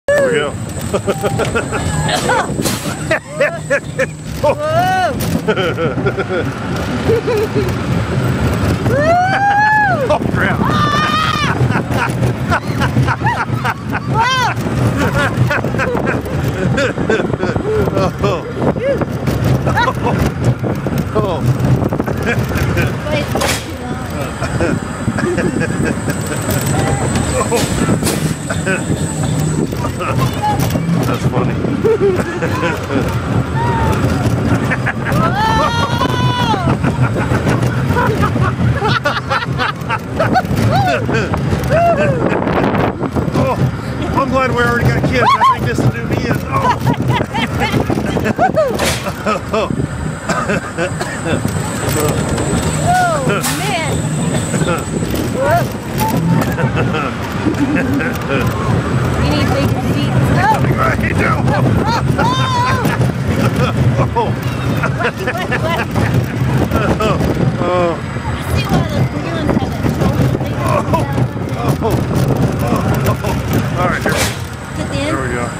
Spring Break 2013 Pigeon Forge Speedway Draft 01
Nascar Speedpark mini coaster

0:43 "I'm glad we've already got kids... I think this would do me in!"